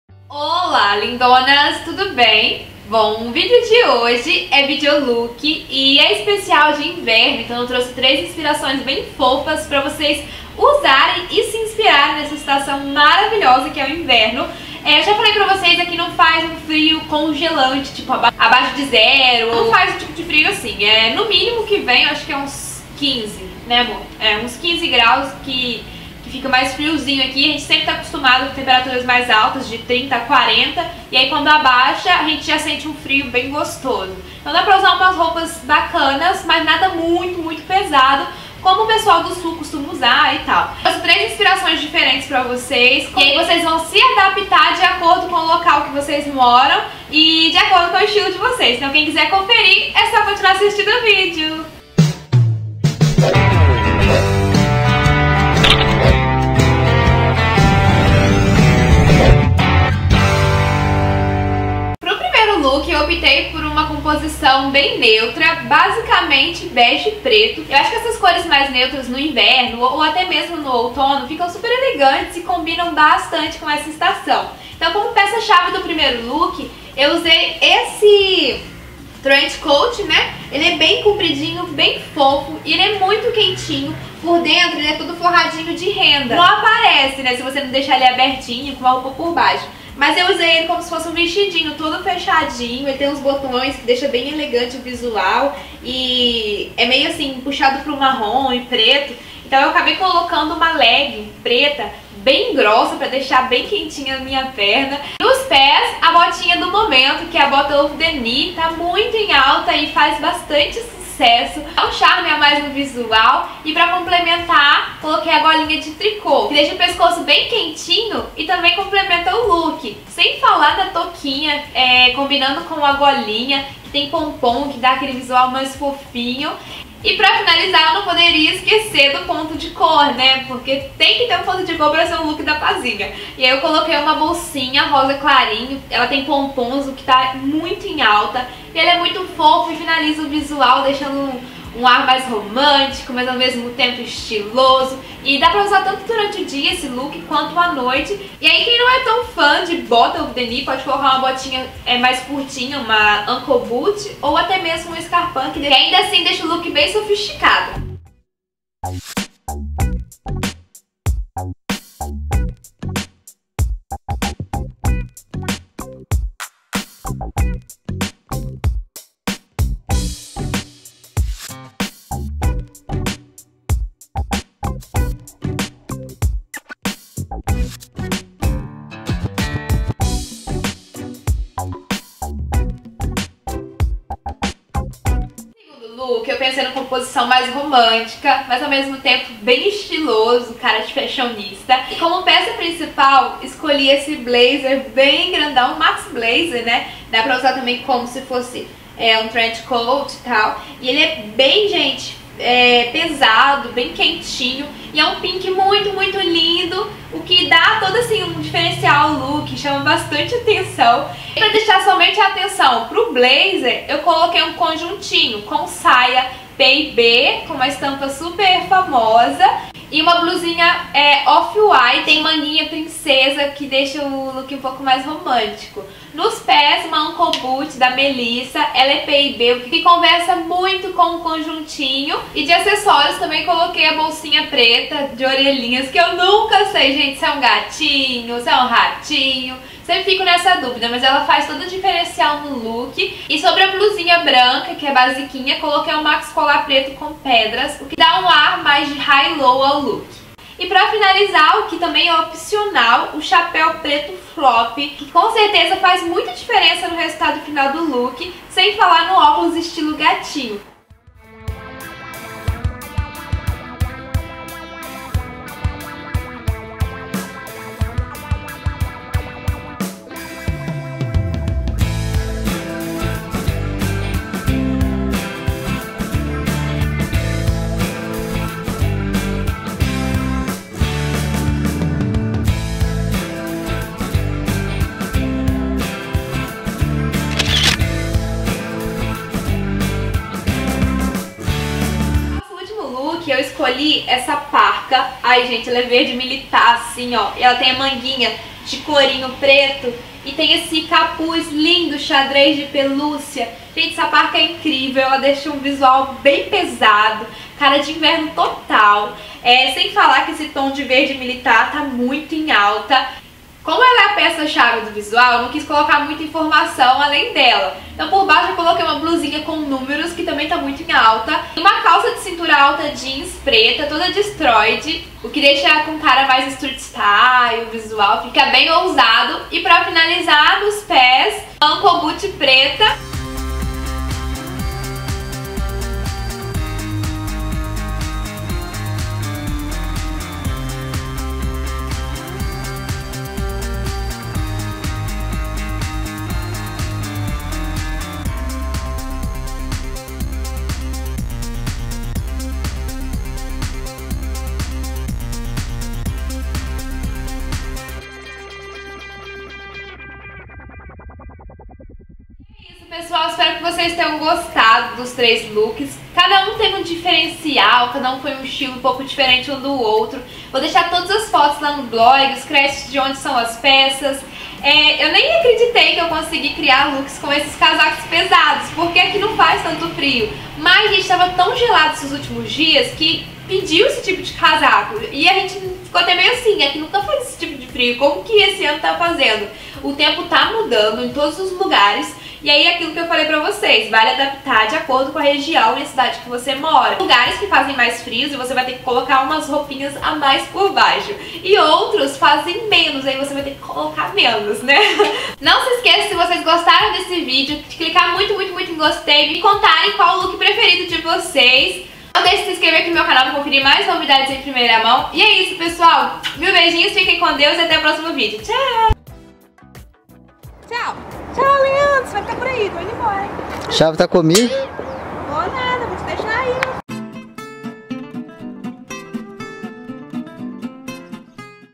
3 Looks de inverno 2015 | Paloma Soares!
Esse é um vídeo de 3 looks de inverno 2015. Vale a pena conferir! Paloma Soares

-snapchat palomasoaresg

Assista em HD. É só clicar na qualidade de imagem e colocar 1080 p HD